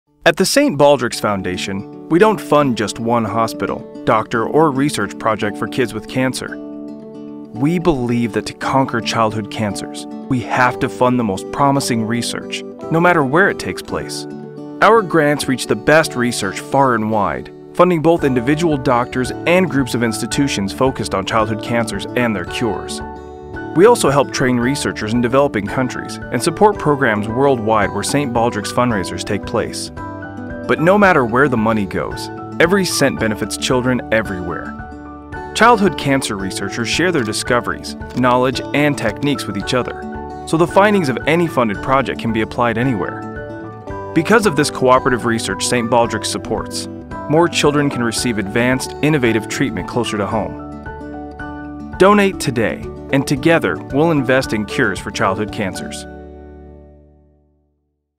St. Baldrick's Foundation - Research
At the St. Baldrick’s Foundation we believe that to conquer childhood cancers we have to fund the most promising research, no matter where it takes place.

Our grants reach the best research far and wide, funding both individual doctors and groups of institutions focused on childhood cancers and their cures.

Every cent benefits children everywhere. Donate today, and together we'll invest in cures for childhood cancers.